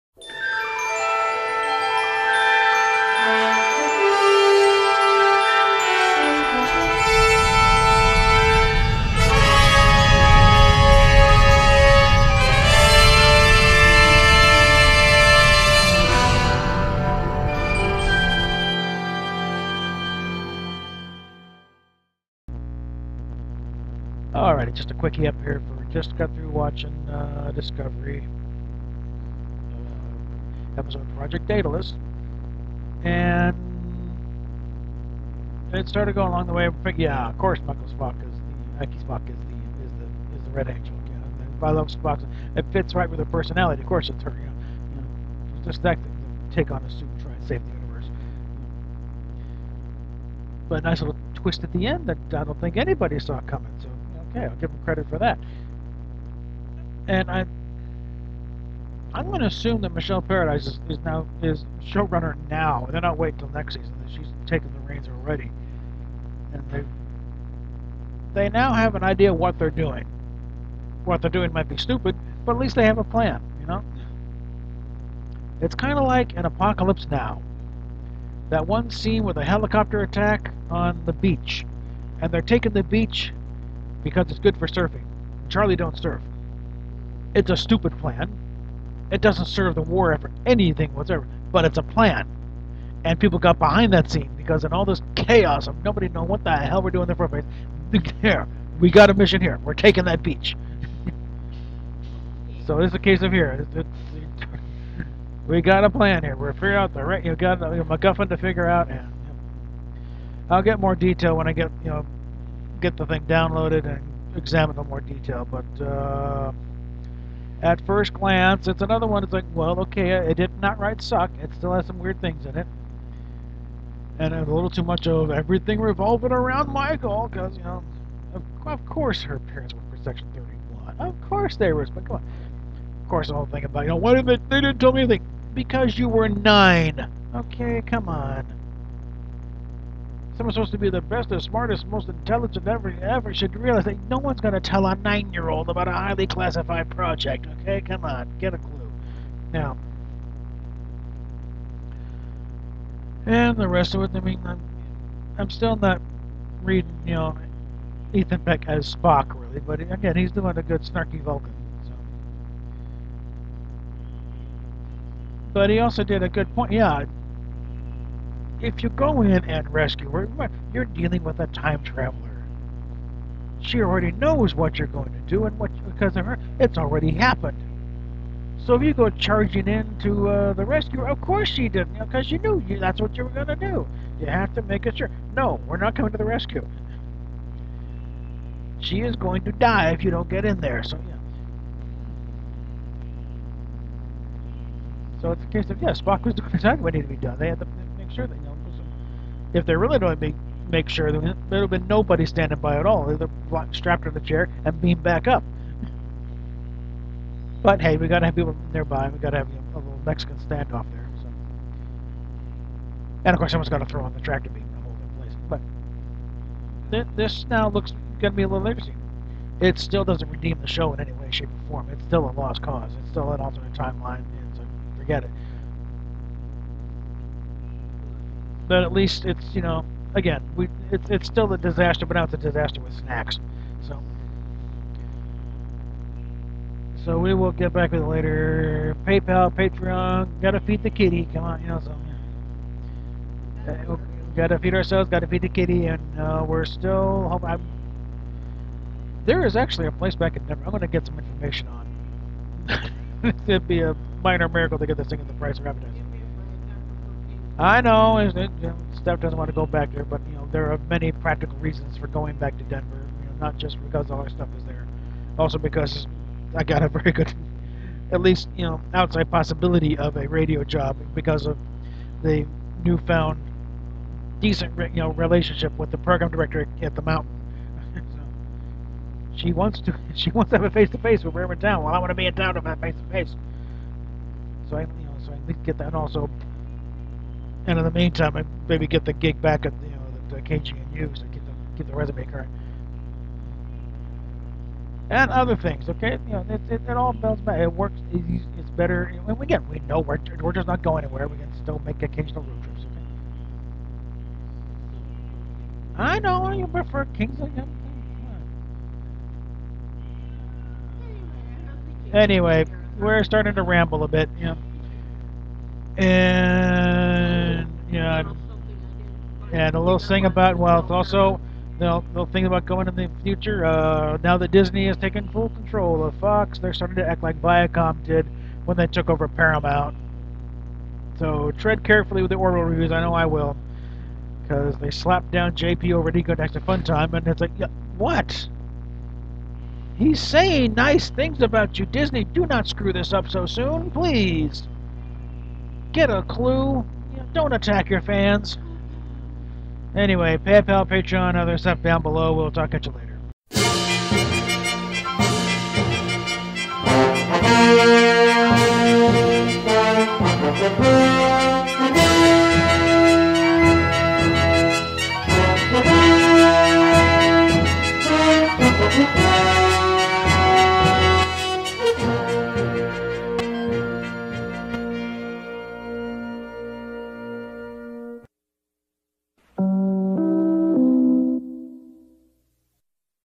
STAR TREK DISCOVERY - THE RED ANGEL - QUICK REACTION
IGNORE MY STATEMENT AT THE BEGINNING, THIS IS THE REVIEW FOR "RED ANGEL".  All I can say is that they're all starting to blur together into one big ball of suck.